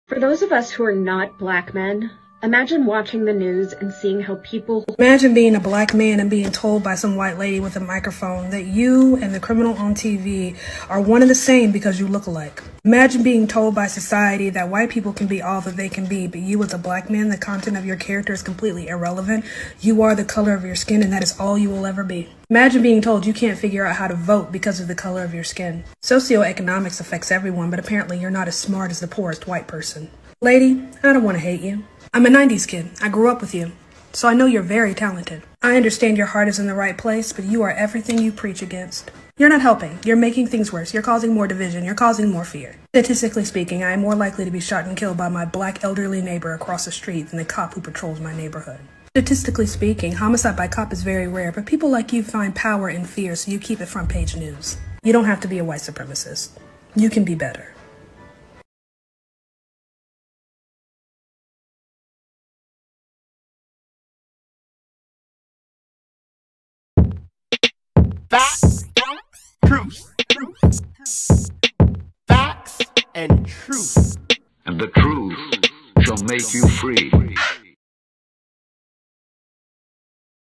Alyssa Milano Shut Down By Awesome Black Woman Speaking TRUTH!
Woman expresses her frustration with liberal lies.